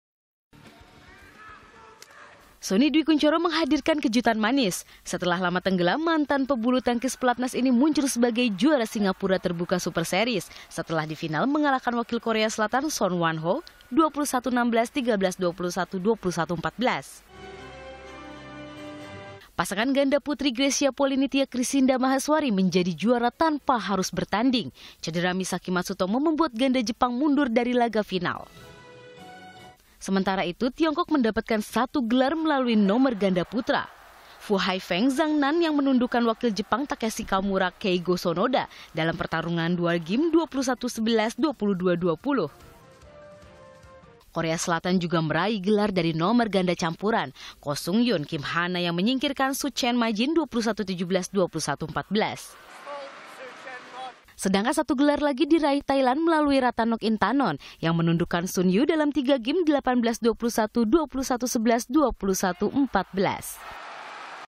Sony Dwi Kuncoro Juarai Singapura Terbuka
Indonesia meraih 2 gelar di Singapura terbuka Super Series melalui tunggal putra, Sony Dwi Kuncoro,dan ganda putri, Greysia Polii dan Nitya Krishinda Maheswari.